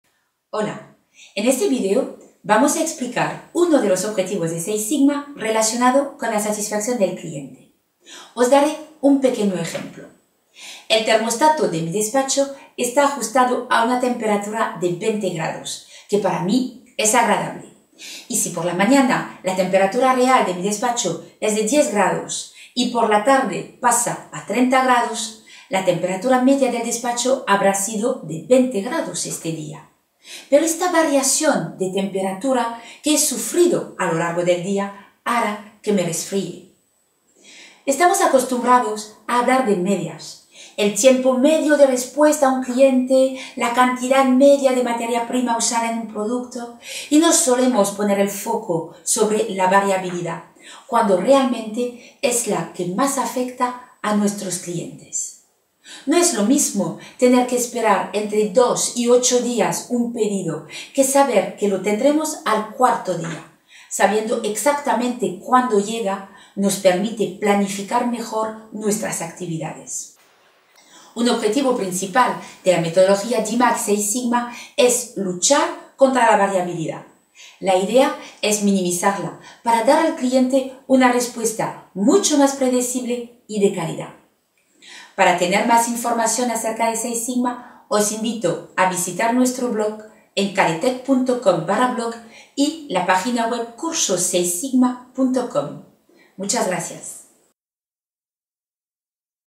Seis Sigma: El problema está en la variabilidad
Breve introducción a la metodología Seis Sigma: Como decía el Sr. Taguchi, "la variabilidad es el enemigo número uno de la calidad".